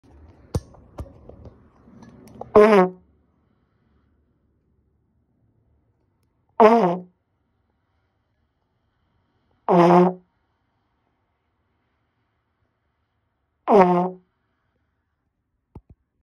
Honking the trombone
When I was in third grade there was a girl and goodness I wish I remembered her name who was a little bit older than me who would literally glare her trombone and you could hear it from a mile away. This is my attempt at doing just that.￼